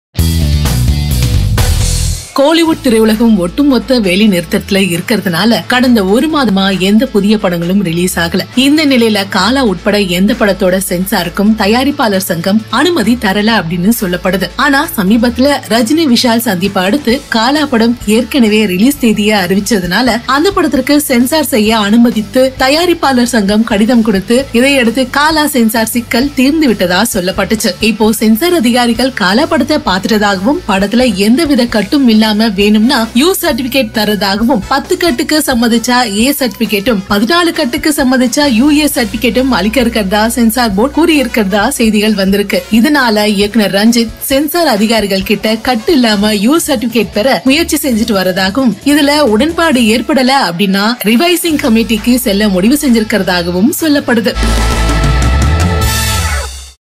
Rajinikanth's Kaala Given U/A Certificate - Releasing on April 27 2018
Rajinikanth and Pa Ranjith film Kaala censor details revealed it seems. Film Chamber of Commerce has given a NOC for the Censor Certification of Kaala. The movie will be certified U/A it seems and is expected to release on April 27.